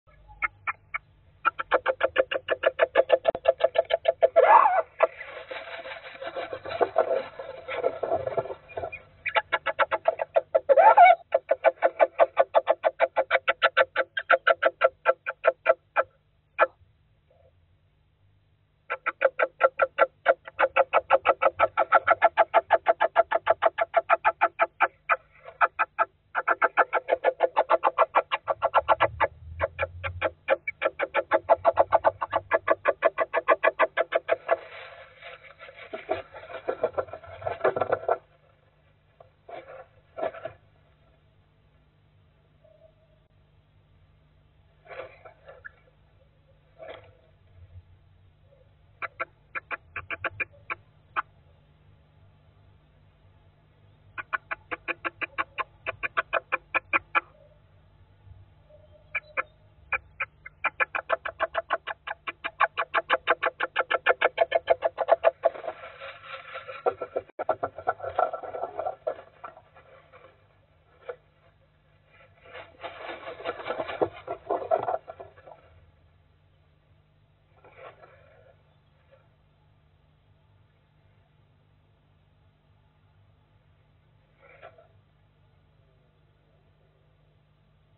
Minnie Minerva finds her nest
Minnie Minerva is a Little owl. Minnie has just found the nest box I made for her... and I think she likes it!